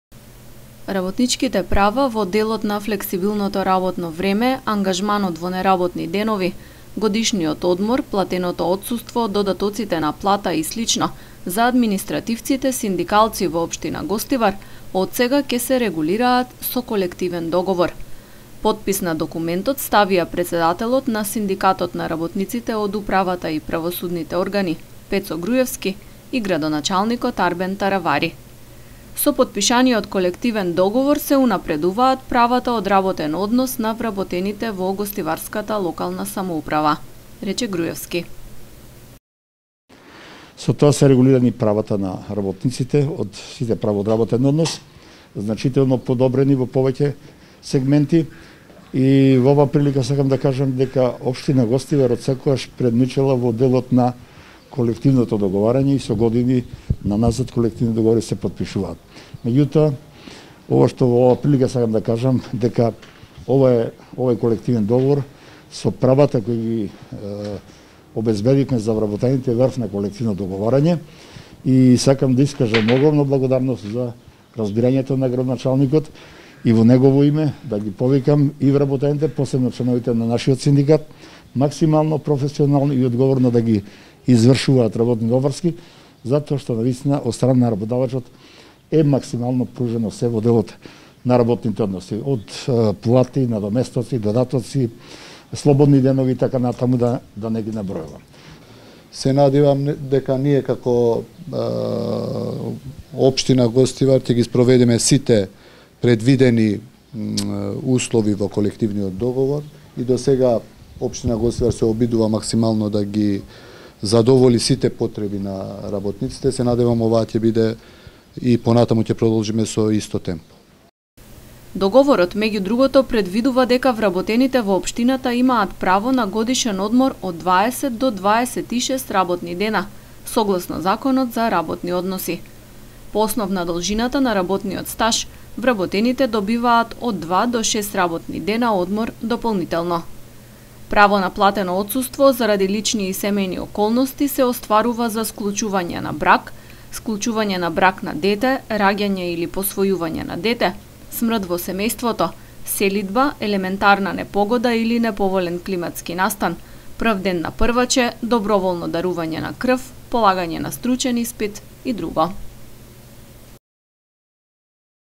Потпишан договор меѓу УПОЗ и Општина Гостивар
Синдикатот на работниците од управата, правосудните органи и здруженијата на граѓани и Општина Гостивар потпишаа колективен договор за регулирање и унапредување на правата на вработените во општината.